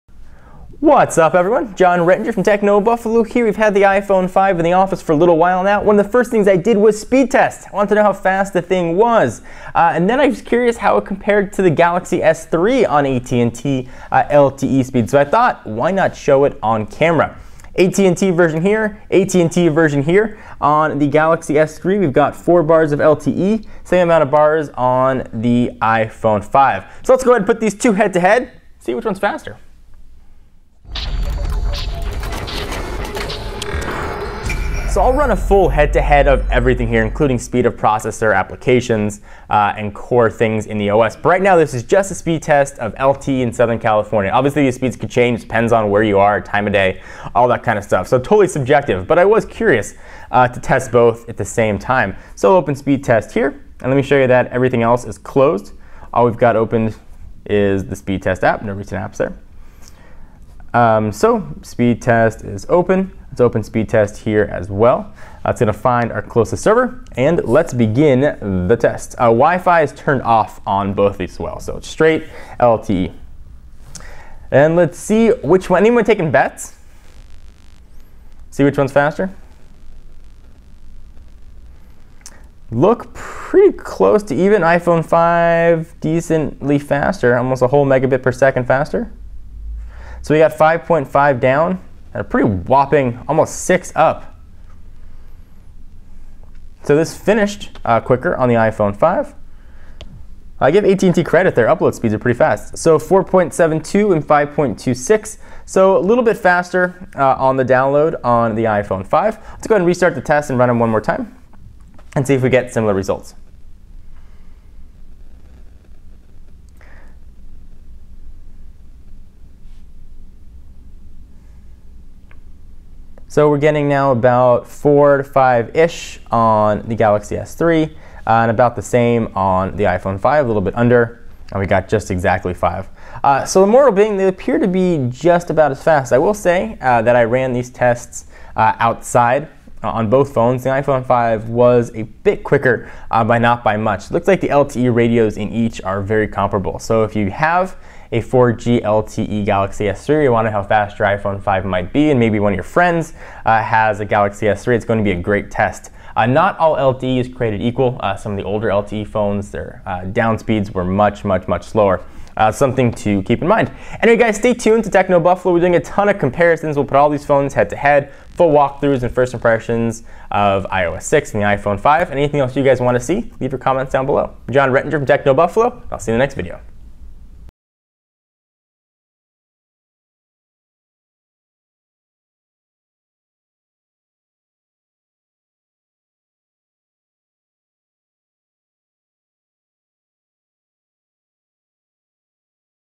iPhone 5 LTE Speed Test vs Galaxy S III
iPhone 5 vs Samsung Galaxy S3 - LTE Speed Test

The iPhone 5 is finally available to the public. And aside from the obvious design changes — thinner, taller, aluminum back — the biggest addition to Apple's new device is the inclusion of LTE. Android users have been experiencing the next generation of wireless technology for some time now, so LTE is nothing new. But for iPhone owners, the step up will be hugely welcome, and put Apple's device in the same league as some of its biggest competitors. So how does it hold up against, say, a Samsung Galaxy S III? Data speeds are hugely important when on the move, so we pitted the two head-to-head to see how their speeds compared.